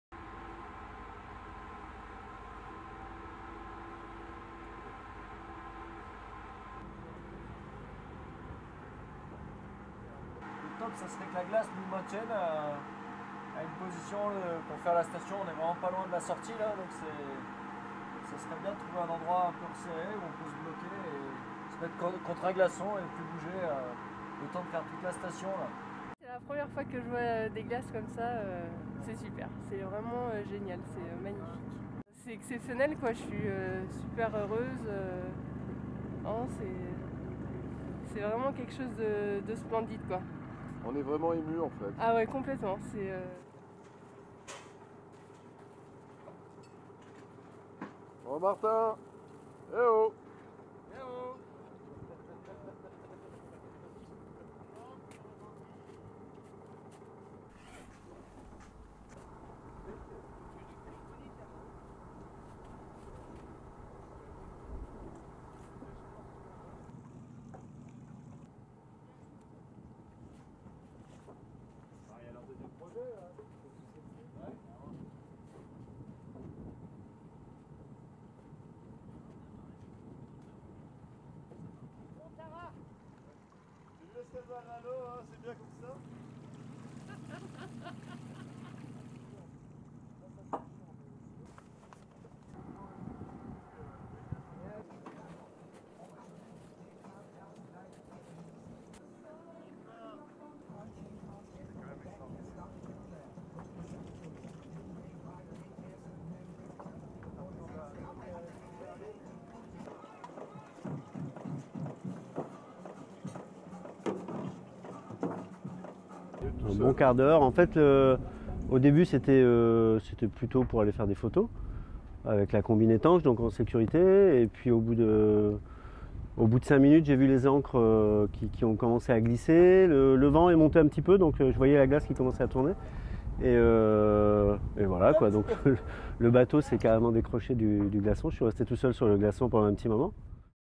On a marché sur la glace - Expédition Tara Oceans Polar Circle - 11 septembre 2013
Quand l'équipage de Tara s'aventure à pied sur la glace.